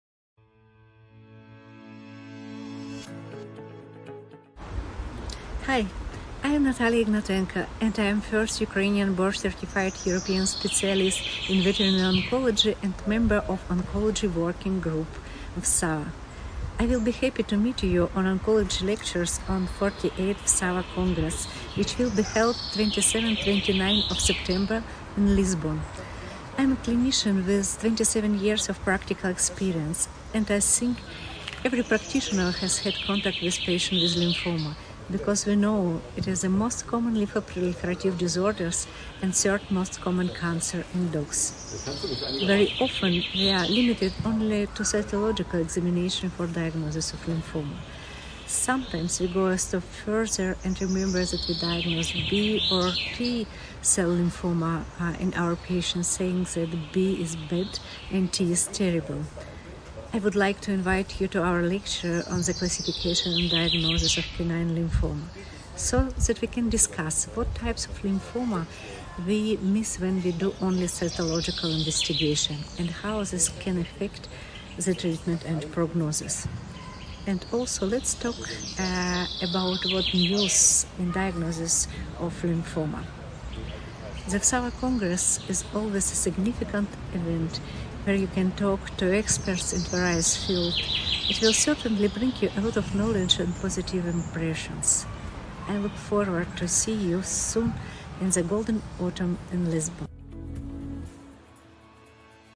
WSAVA 2023 - Dr Natalia Ignatenko - Program Highlight
Dr. Natalia Ignatenko, a member of the Oncology working group of WSAVA, will be sharing insights on the classification and diagnosis of canine lymphoma at WSAVA2023. Expand your veterinary expertise and join us in Lisbon